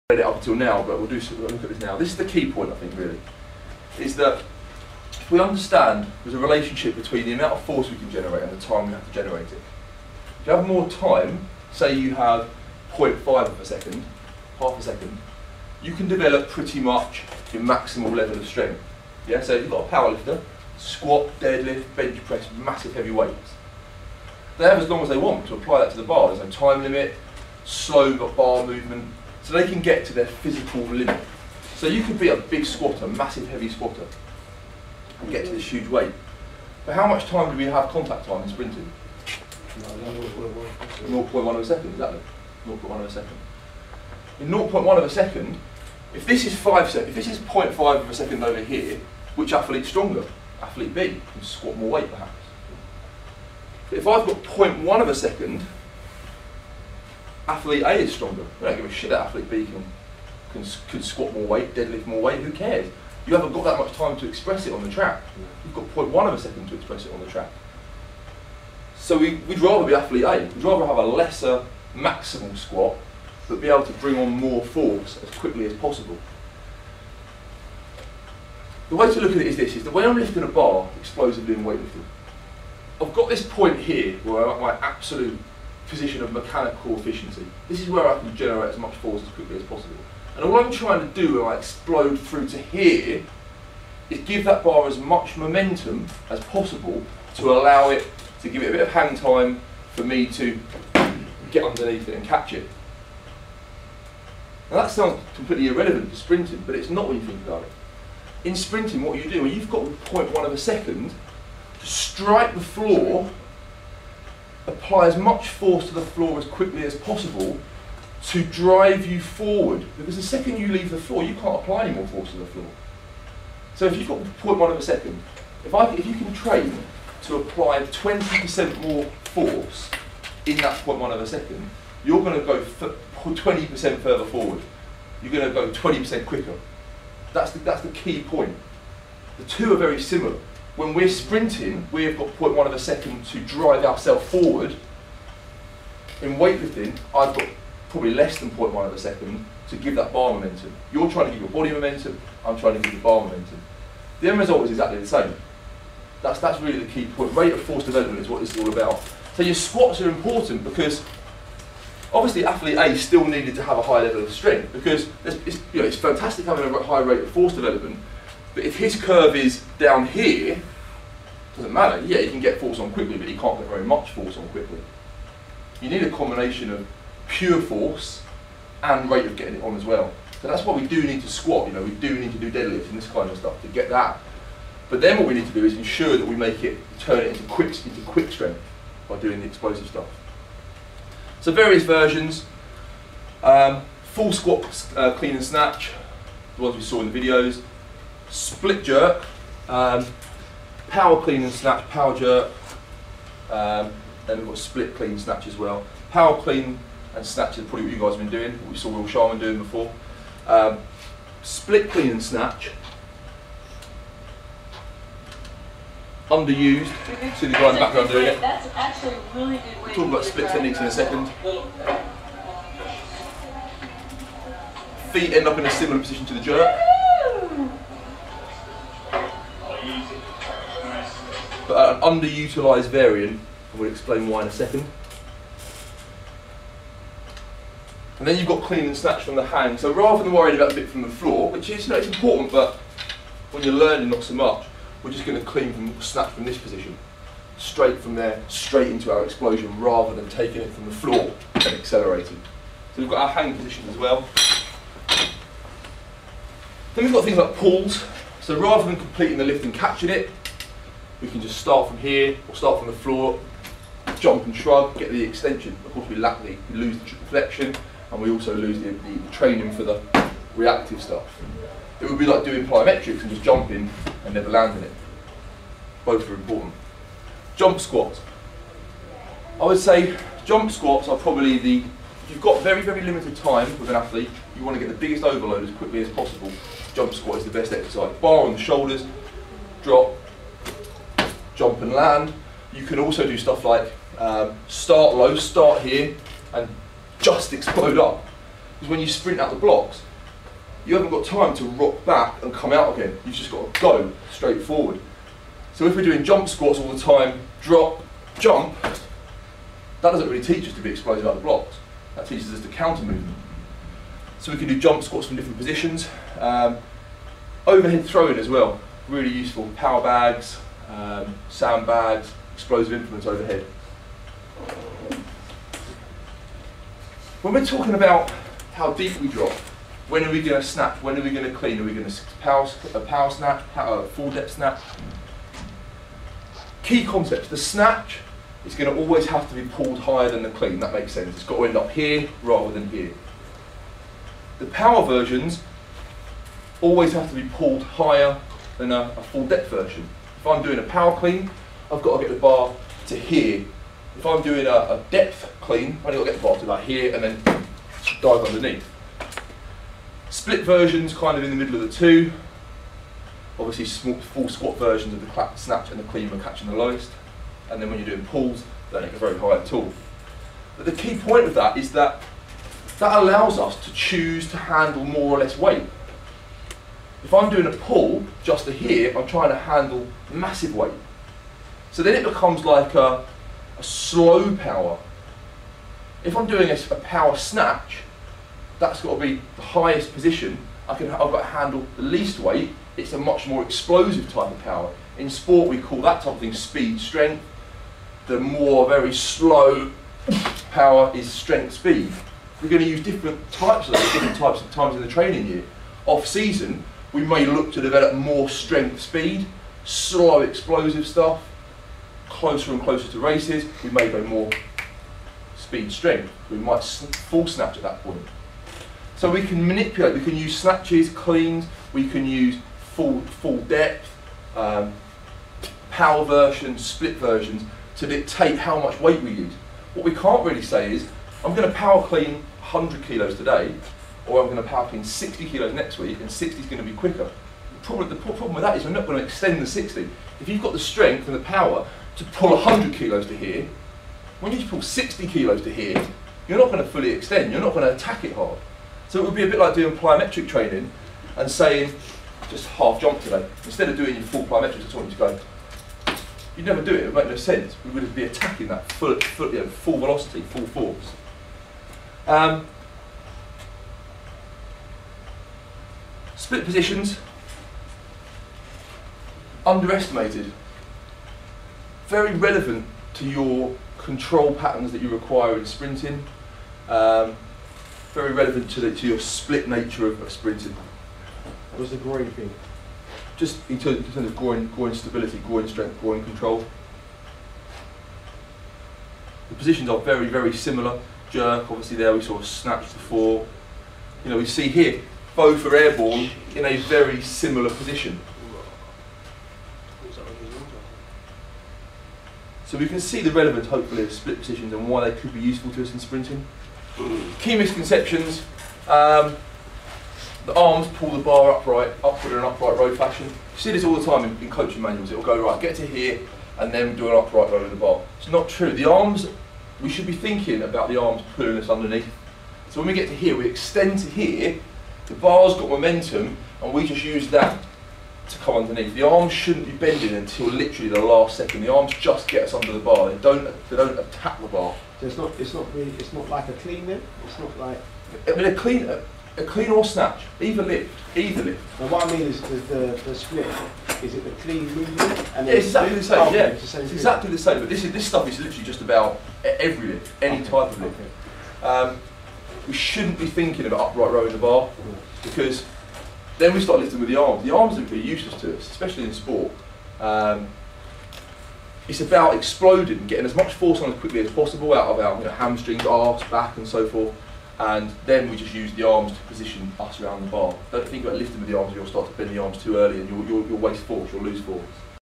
Olympic Weightlifting for Sprinting Part 4
Phil Lectures Top Coach Sprint Squad on why and how Olympic Weightlifting is beneficial to Sprinters.